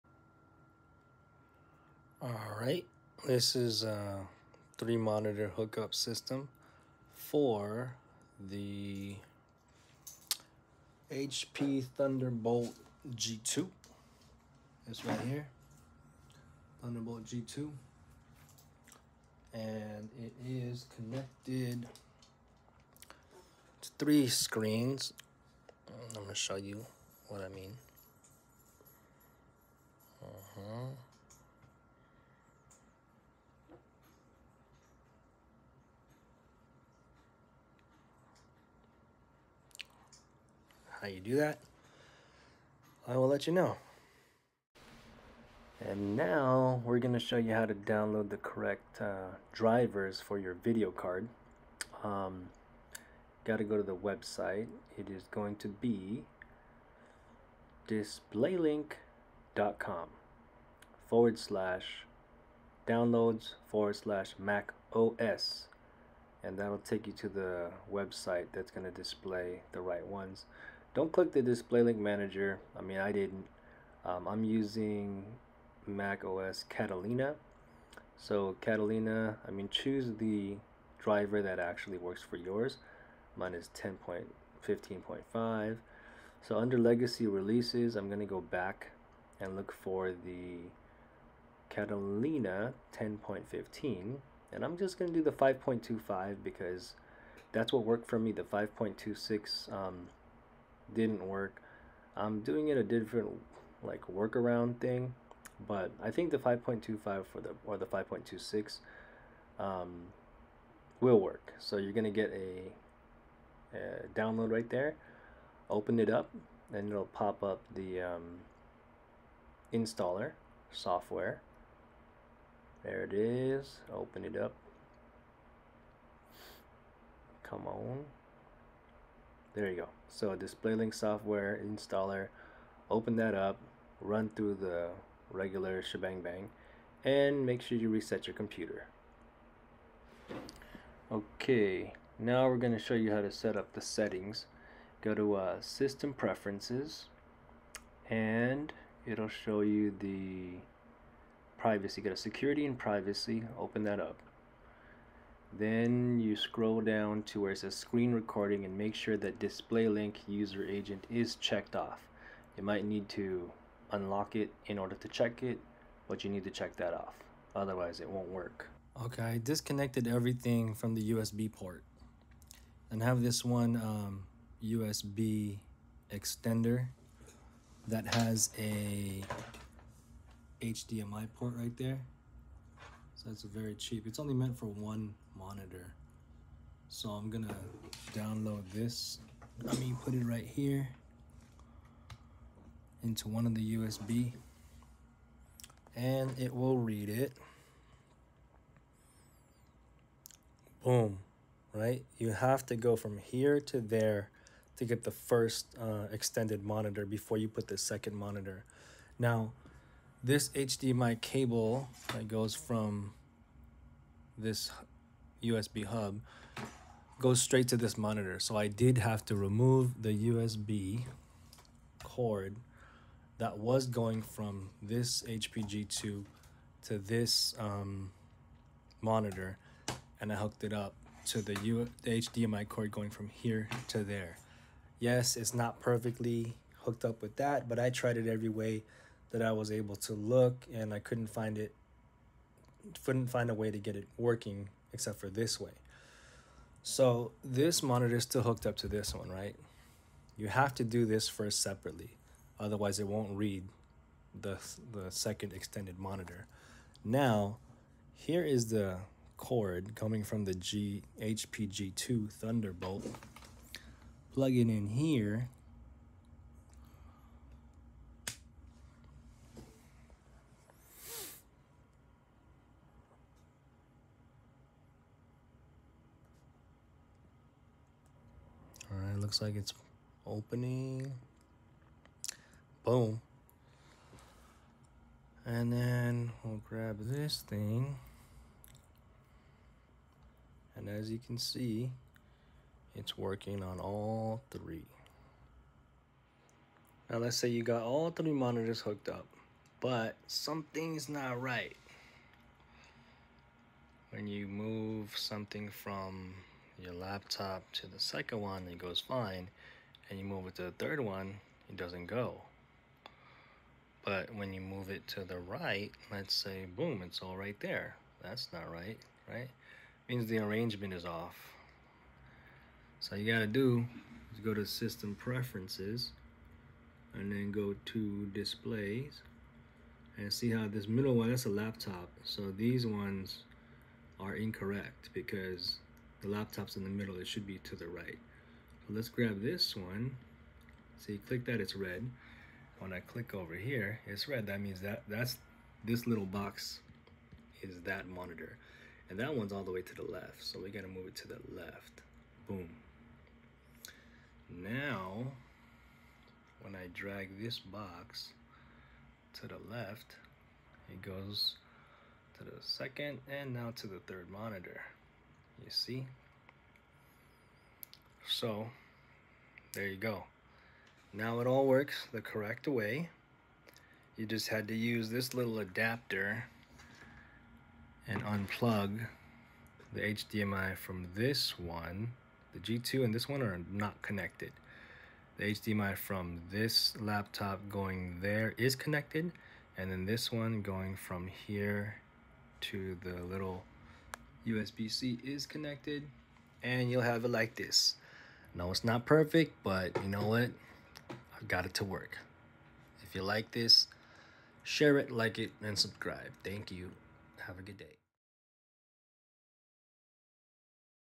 How I Connected my Macbook Air 2020 to My HP Thunderbolt G2 Hub from Work
This video is about How I Connected my Macbook Air 2020 to My HP Thunderbolt G2 Hub from Work. So I work from home now and I was given two monitors along with a central hub which happens to be the HP Thunderbolt G2 which is very useful because you can plug in a bunch of stuff like an external keyboard an external mouse to monitors a webcam and everything. I decided to figure out a way that I could remove my HP laptop and use my personal MacBook Air 2020. I searched YouTube and tried to figure out how to connect it but alas none of these ways worked. I combined some methods and this is how I was able to get mine to work. It forced me to remove the second monitor from the HP Thunderbolt G2 and connect that second monitor to my MacBook directly via a new HDMI cord which went into the HDMI input from my USB hub (directly to my laptop on a USB-C port). Once that monitor was connected then I was able to connect the HP Thunderbolt G2 cord to the other USB-C port on my MacBook. This enabled both monitors to become extension monitors from my MacBook. In the end I was able to use 3 monitors, this is including my MacBook Air laptop monitor. Hopefully this helps someone out there. This is the way I used my work from home equipment supplied by the company for personal use after hours. Good luck and let me know if you have any questions! Please no negative comments but if there are Oh well Que Sera Sera!

Click this link below to get an adapter like the one I used in this video: